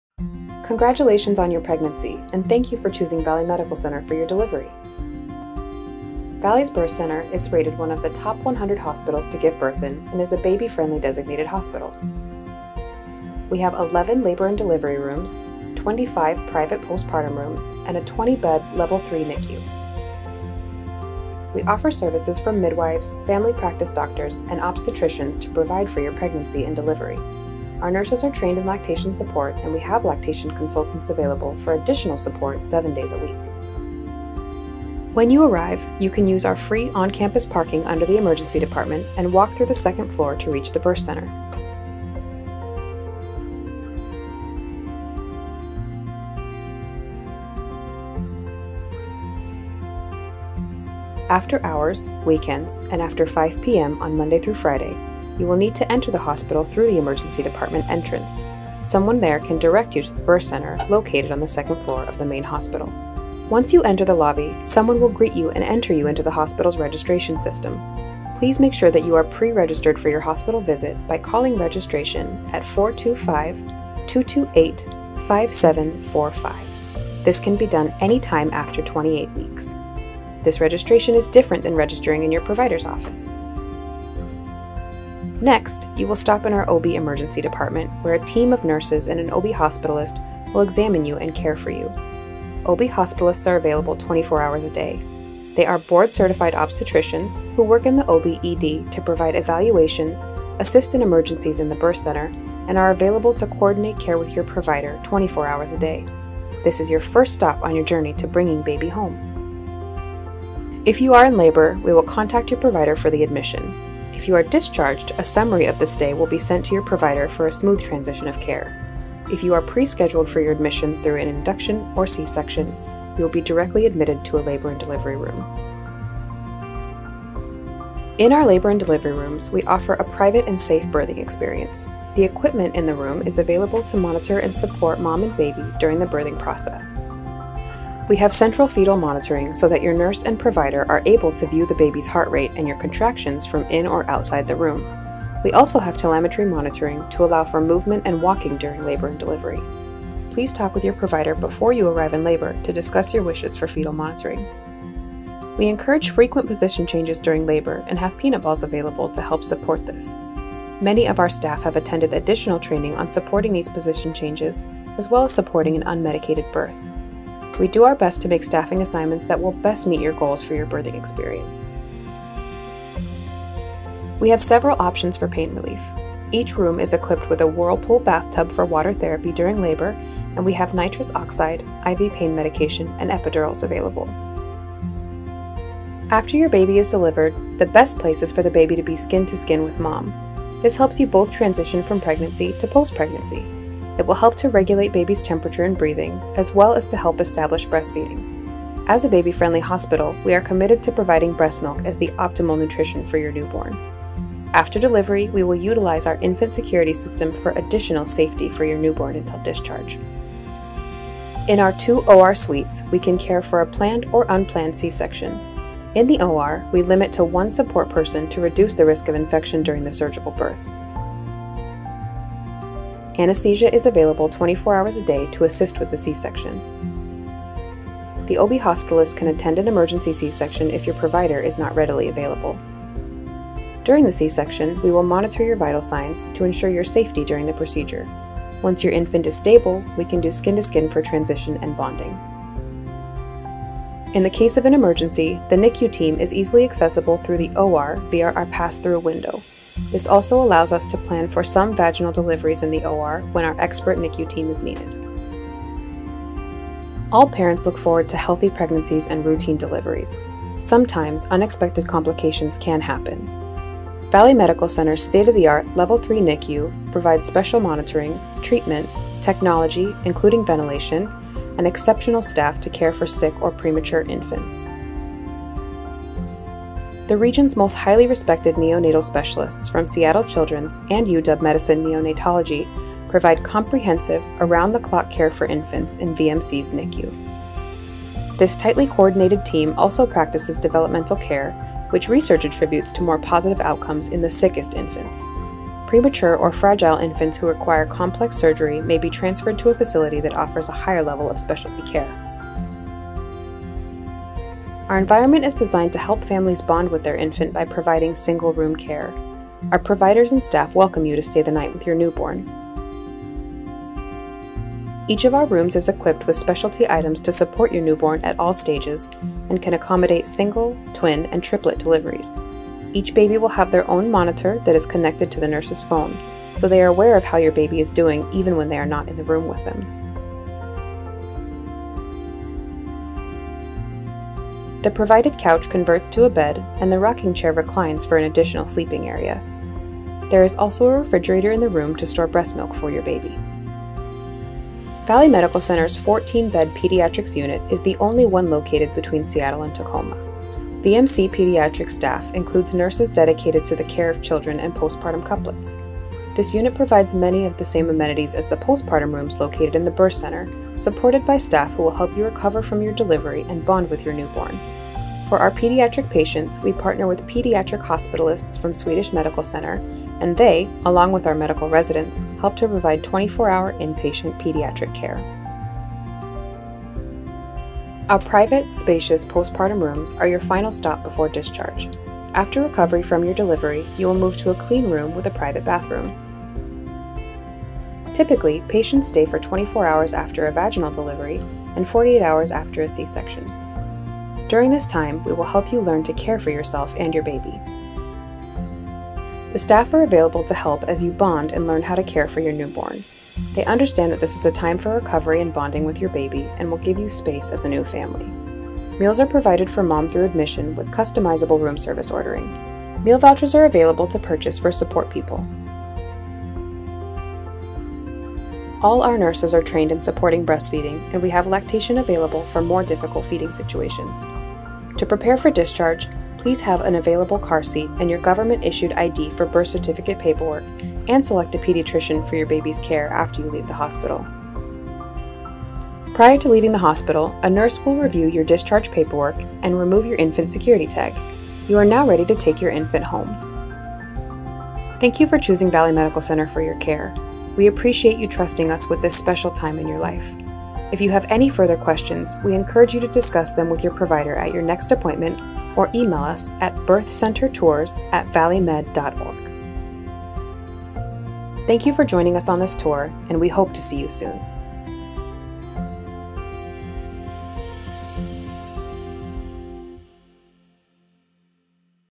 Birth Center Tour 2020 - Valley Medical Center
To watch with captions, click [CC] in the video play bar.

Join us on a tour of Valley Medical Center's Birth Center, a warm and inviting place to bring baby into the world. With a state-of-the art level 3 NICU and providers on call around the clock, we are here to care for you like family, no matter what, no matter when.